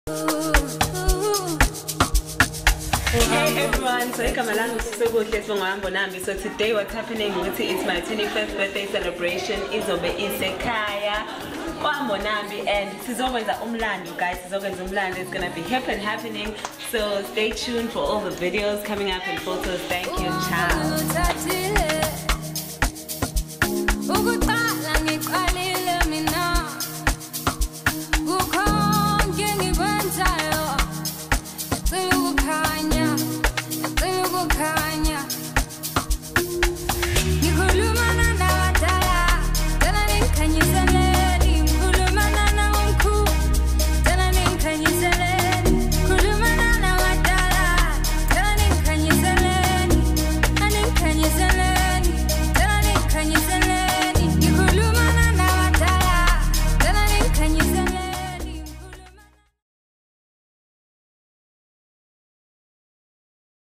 25th Birthday Celebration Highlights
Sfisokuhle Mbonambi's 25th Birthday Celebration Highlights... Shot and Edited by Uhambo Lusaqhubeka Photography.